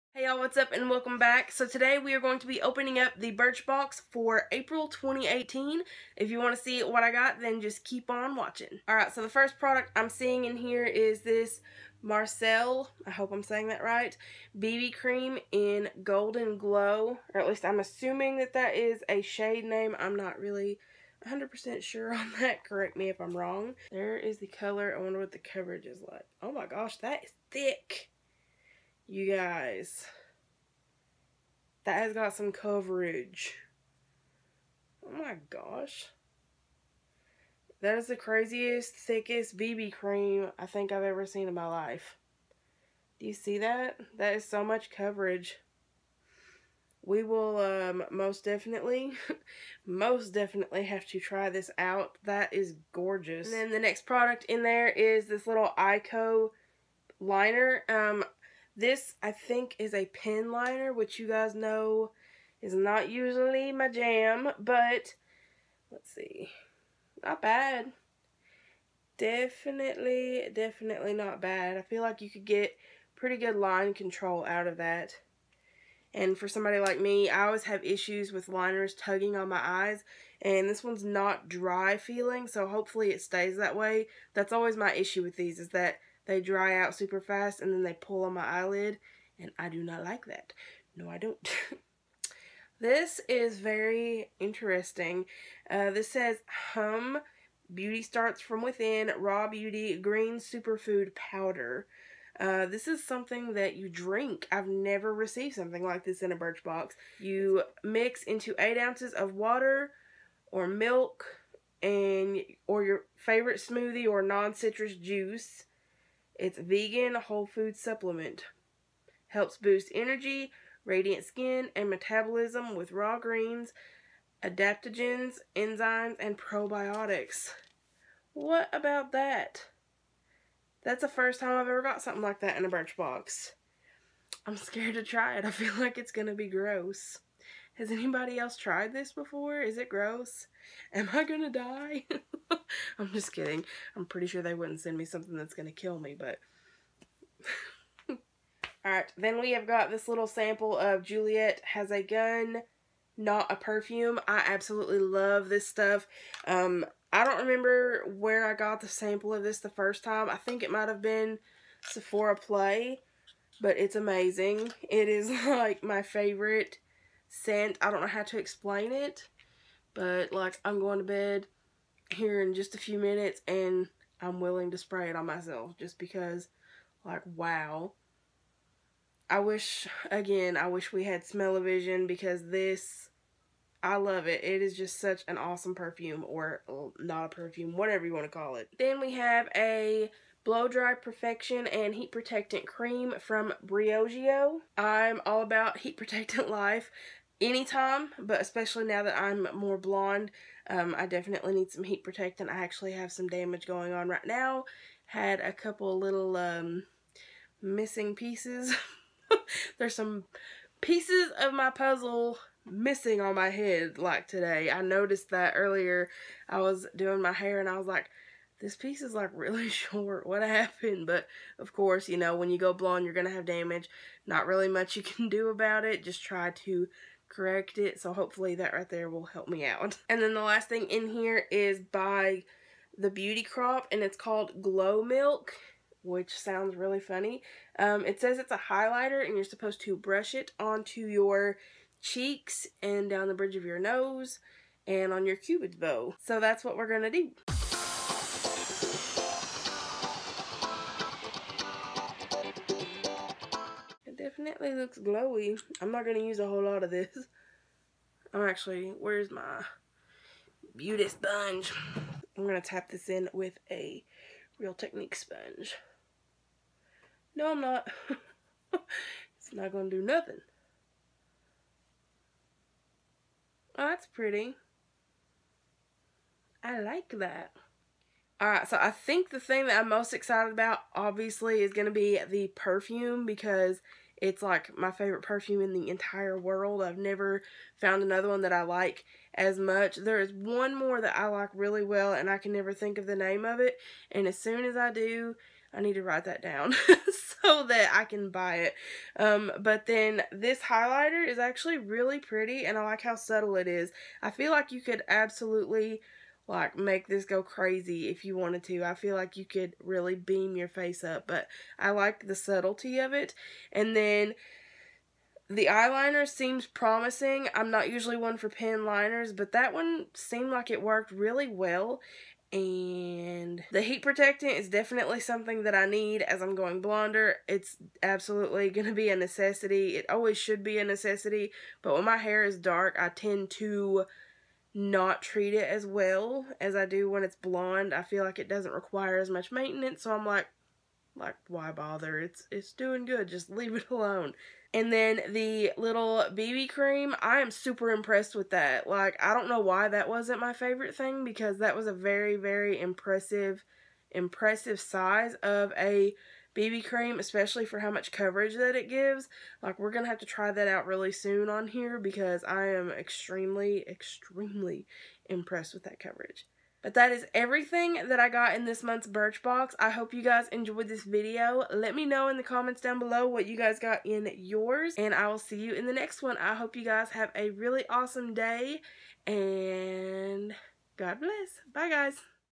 April 2018 Birch Box
So I have been loving these birch boxes lately guys! I am so happy I signed back up and gave it another shot!
I'm selling makeup! To purchase click the link here👇🏻